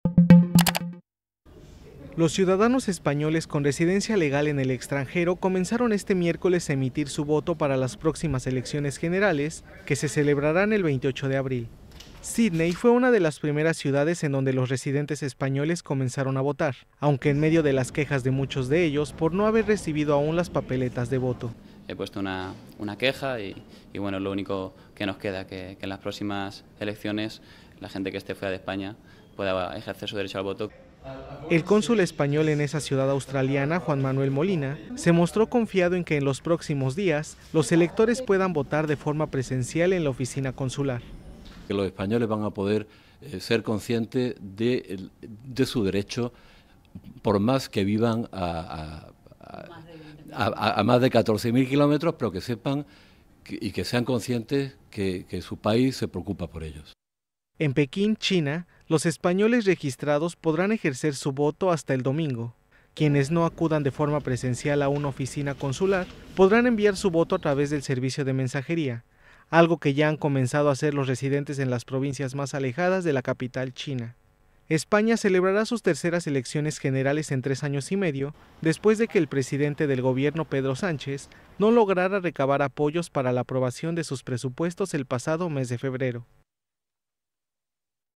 Españoles votan en el extranjero entre quejas por no haber recibido las papeletas
Sídney/Pekín 24 abr (EFE).- (Imágenes: Rocío Otoya) Españoles residentes en Sídney comenzaron hoy a votar para las elecciones generales del domingo en el consulado de esta ciudad australiana, en medio de las quejas de muchos de ellos por no haber recibido aun las papeletas de voto.

Imágenes: Rocío Otoya y Javier Castro Bugarín

Edición y locución: Octavio Guzmán